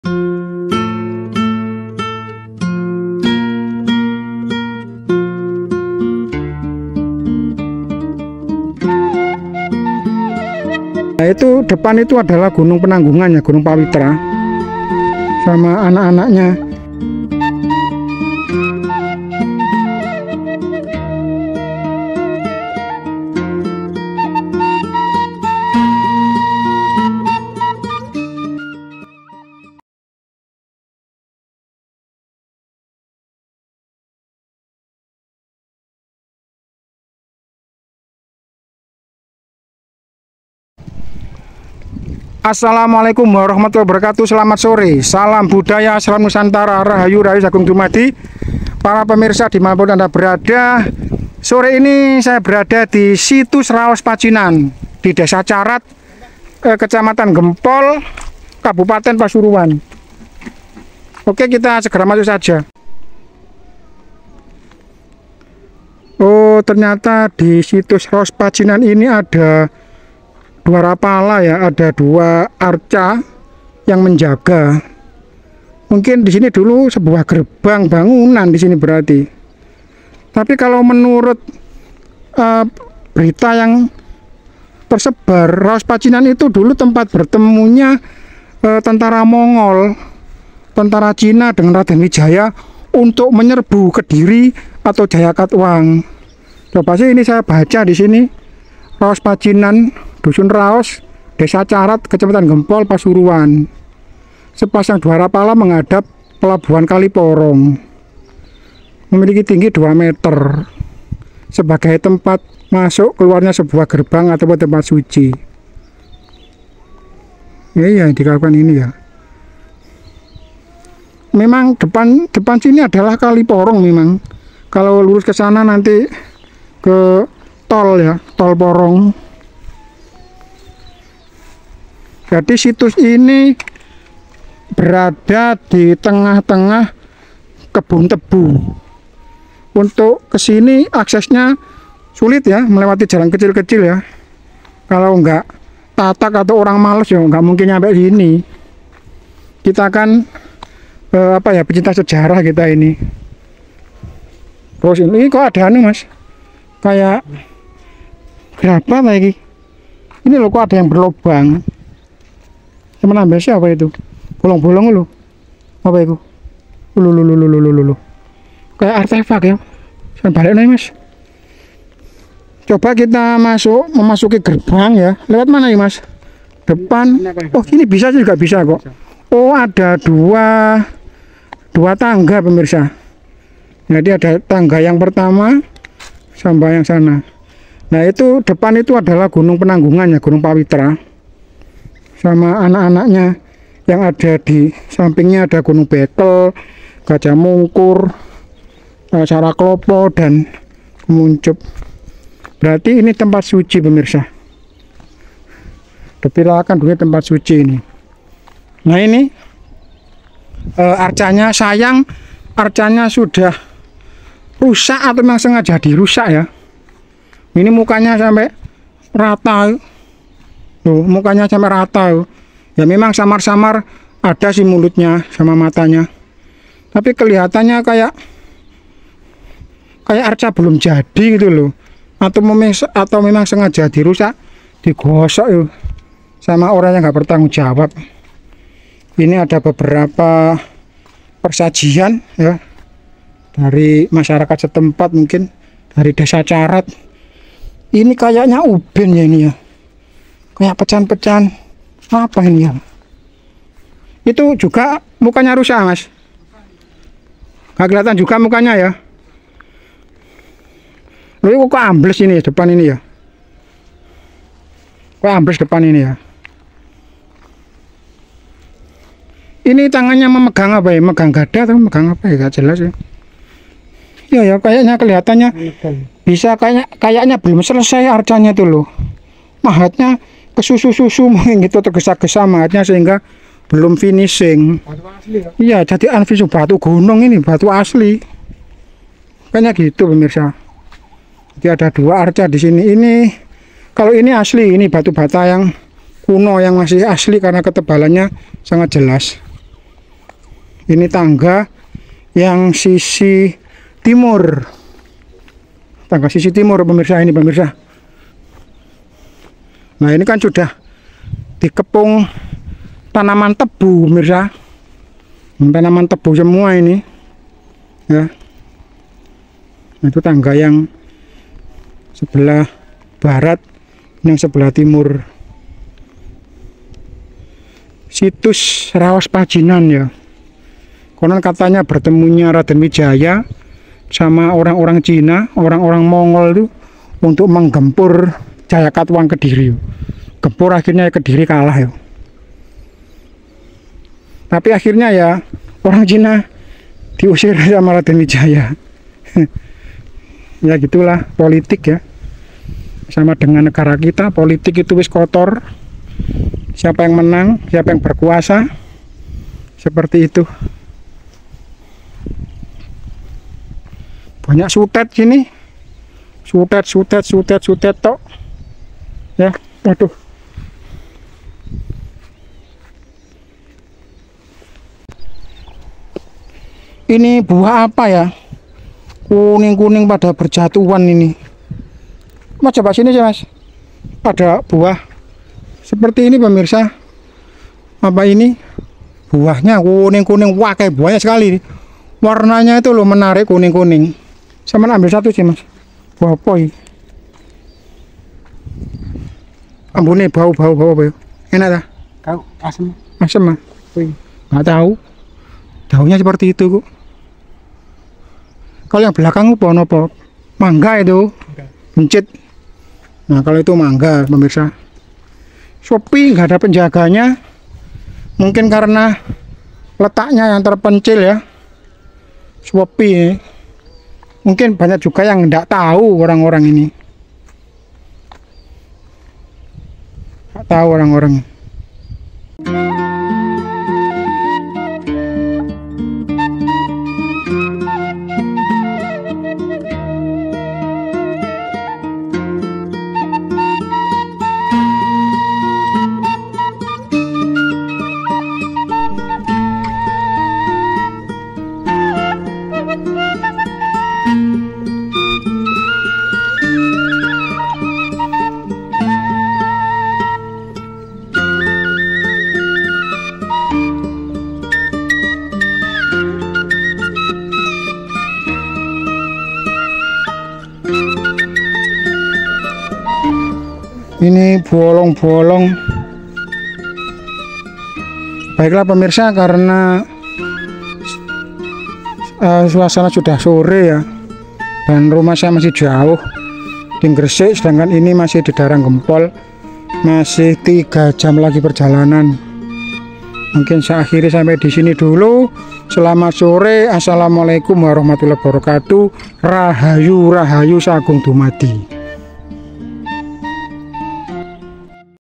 Raos Pacinan Tempat Bertemu nya Raden Wijaya Dan Pasukan Mongol Untuk Menyerbu Kediri Jayakatwang
Carat adalah sebuah daerah yang terletak dibibir Sungai Porong ,disinilah dulu ada sebuah Tempat suci tempat pelarian Raden wijaya saat diserang oleh Kediri /Prabu Jayakatwang saat menyerang Singosari
Ditempat ini pula Raden Wijaya dan Pasukan mongol bertemu dan mengatur siasat penyerbuan terhadap Prabu Jayakatwang Kediri yang saat itu menguasai Kerajaan Singosari.
Setelah penyerbuan ini sukses carat berubah menjadi sebuah dermaga/Pelabuhan
yangng banyak dihuni oleh orang Cina dan Mongol, maka nya tempat ini dinamai dengan RAOS PACINAN Yang berasal dari kata Desa Raos yang banyak dihuni orang Cina. Wallohu aklam